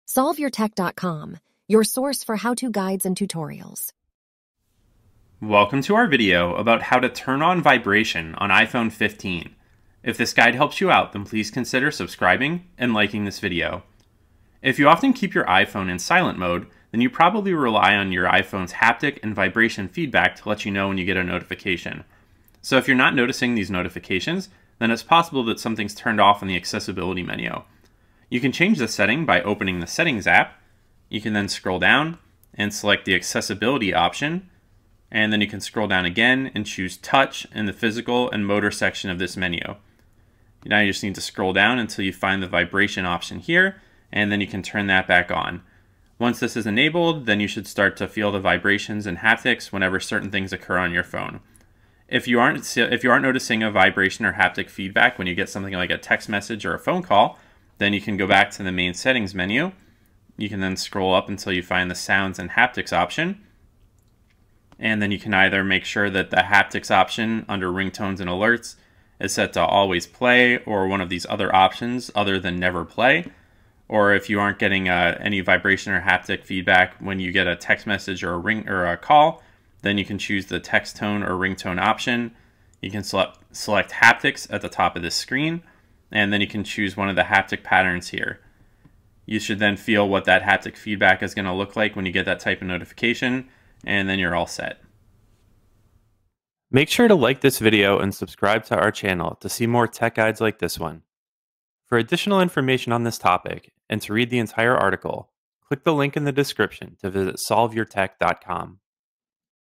How to Turn on Vibration on iPhone 15
This video will show you how to turn on vibration on iPhone 15. Once you complete this tutorial, you will know how to find and change the setting that controls whether or not your phone uses vibration.

You can turn iPhone 15 vibration on with the following steps:

1. Open Settings.
2. Choose Accessibility.
3. Select Touch.
4. Enable the Vibration option.

If your phone isn’t vibrating when you receive certain types of notifications, then you can usually adjust these by going to the Settings - Sounds & Haptics menu and adjusting the related option there.

00:00 Introduction
00:14 How to Turn On Vibration on iPhone 15
01:36 Outro

Related Questions and Issues This Video Can Help With:

- How to Turn On Vibration on iPhone 15 Mini
- How to Turn On Vibration on iPhone 15 Plus
- How to Turn On Vibration on iPhone 15 Pro
- How to Turn On Vibration on iPhone 15 Pro Max